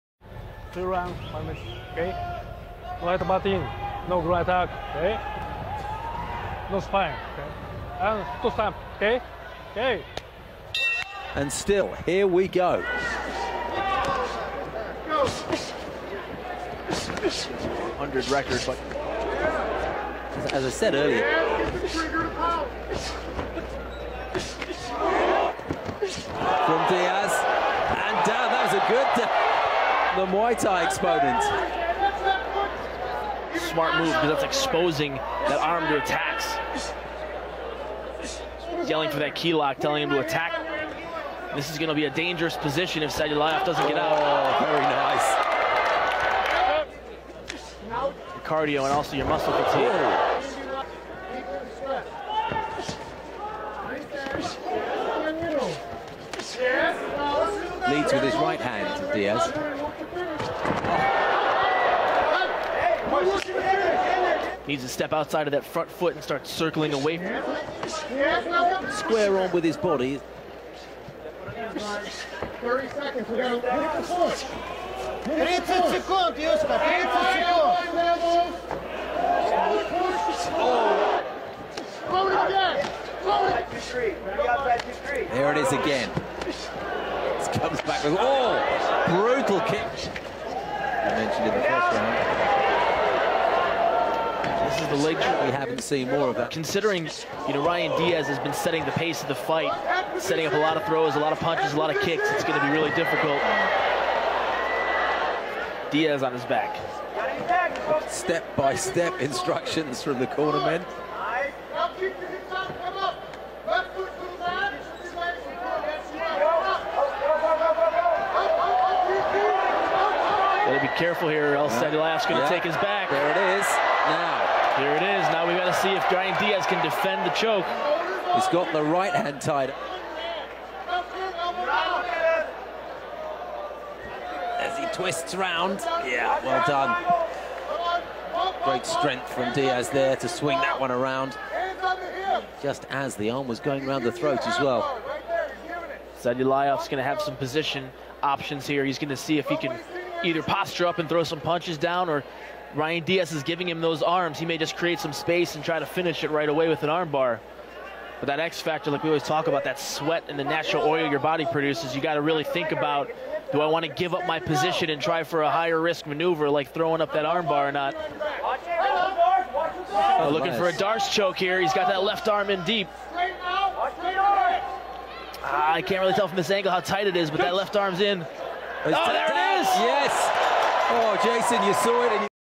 HOT VS COLD \ Yusup Saadulaev VS Ryan Diaz \ Was Wild
HOT VS COLD  This throw  scrap between Yusup Saadulaev and Ryan Diaz was WILD!